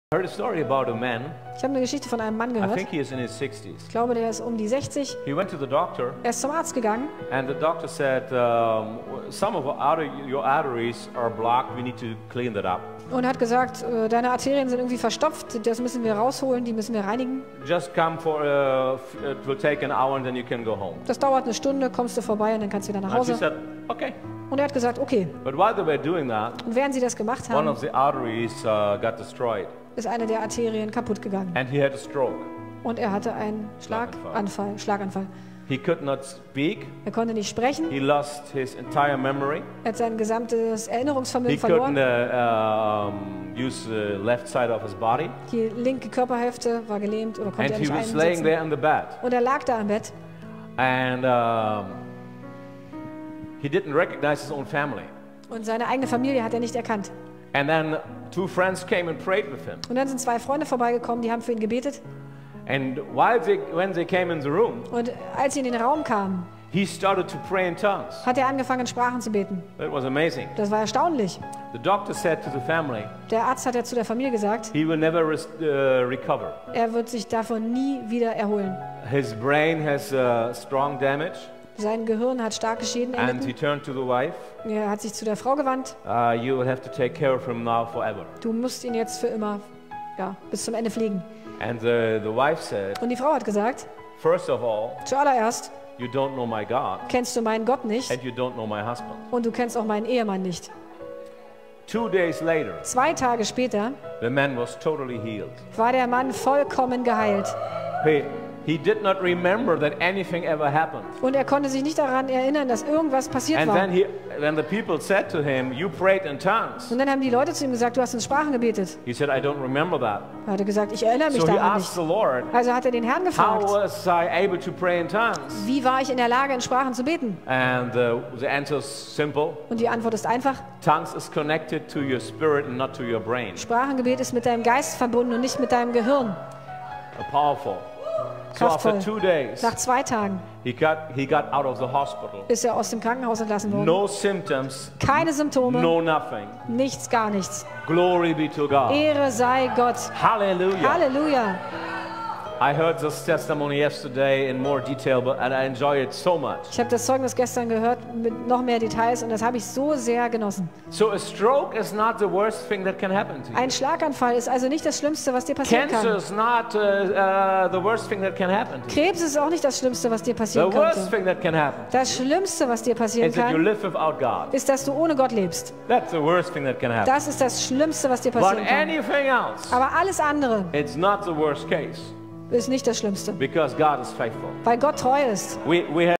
Matthias Schmöcker: "You don't know my God and you don't know my husband!"
Original: Gottesdienst Victory Revival Church Gnade Empfangen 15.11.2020